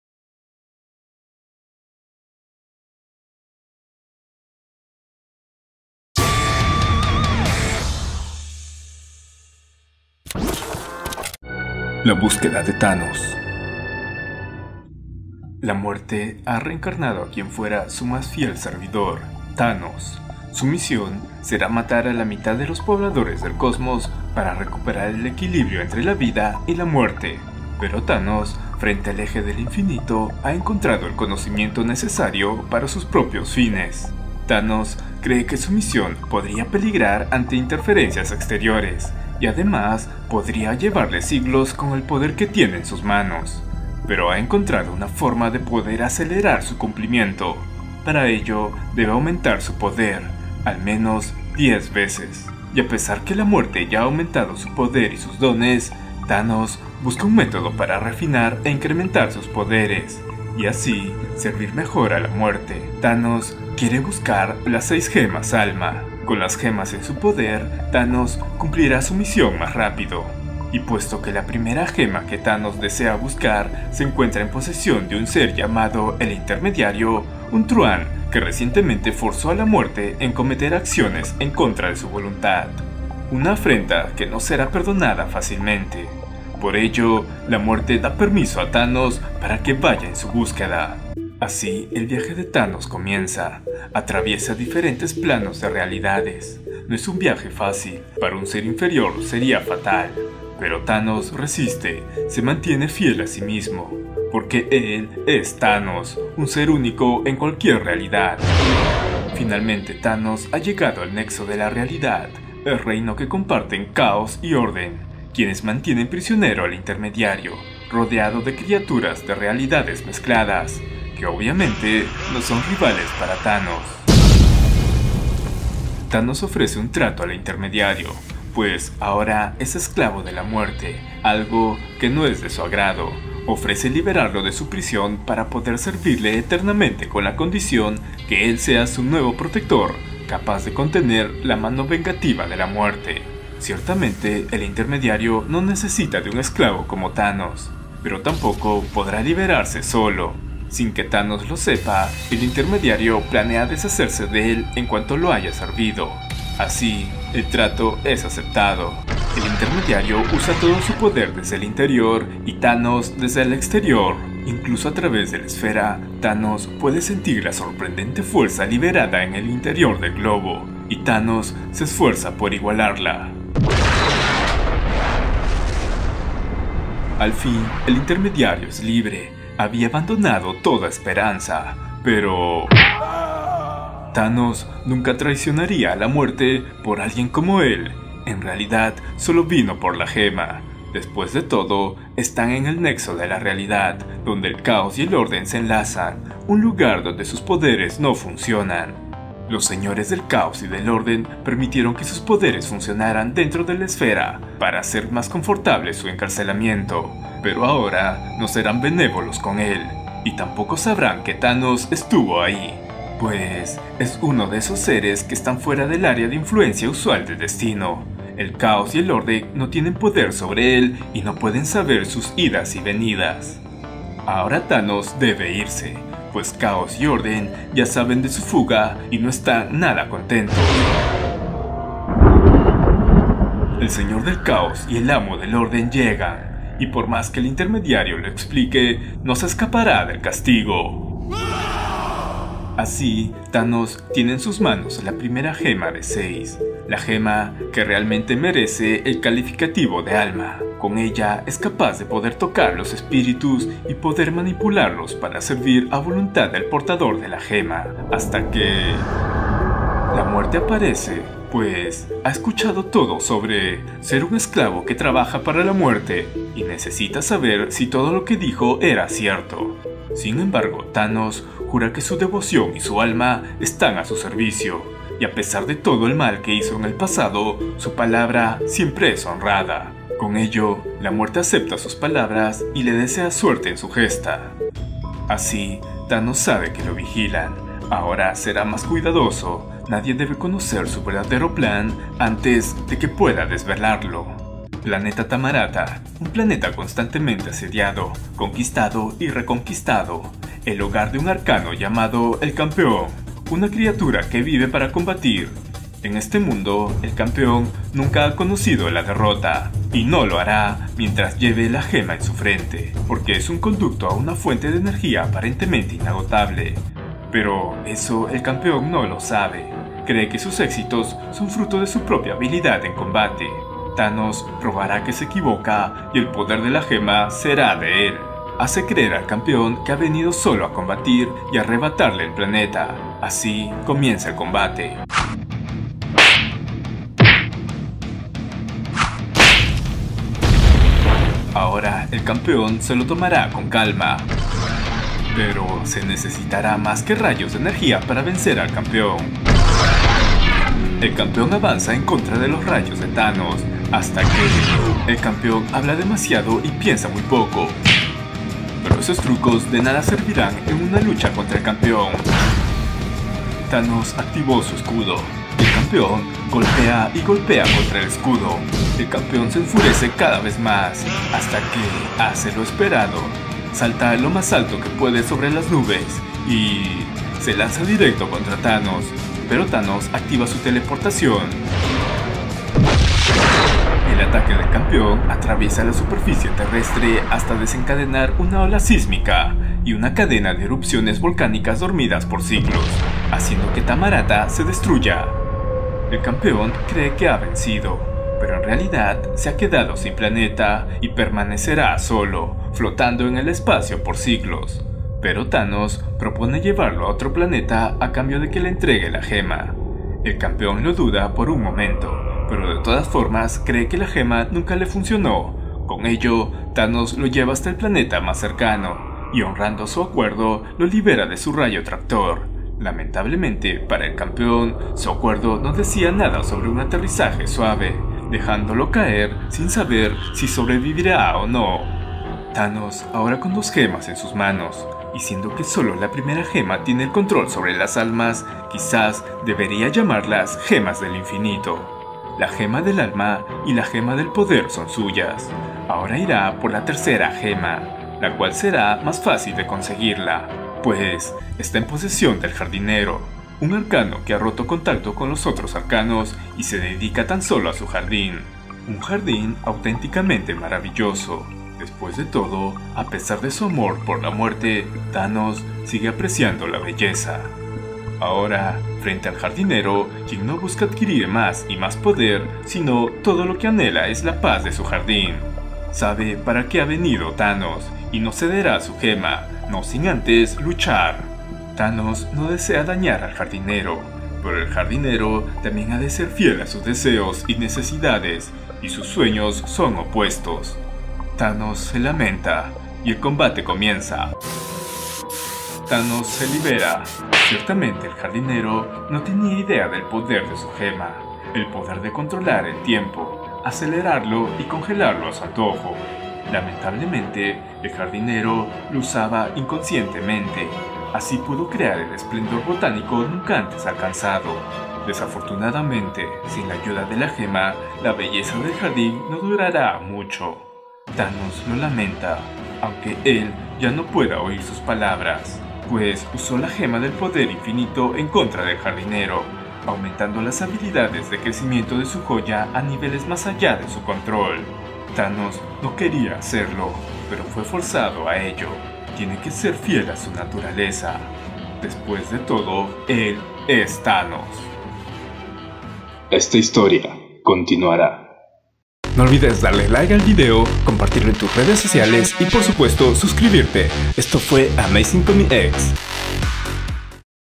THANOS BUSCA LAS GEMAS DEL INFINITO - EL PLAN SECRETO DE THANOS - THE THANOS QUEST #1
EL REGRESO DE THANOS

THANOS ha regresado a la vida por parte de la señora MUERTE.
Con la finalidad de equilibrar el cosmos de los eres vivos y los muertos. La misión de THANOS es matar a la mitad del cosmo.

Pero THANOS, ha encontrado una forma mas eficaz de realizar su misión, pero con un plan secreto. Y la señora MUERTE sin saberlo permite su gesta.

¿Cuál será el plan de THANOS?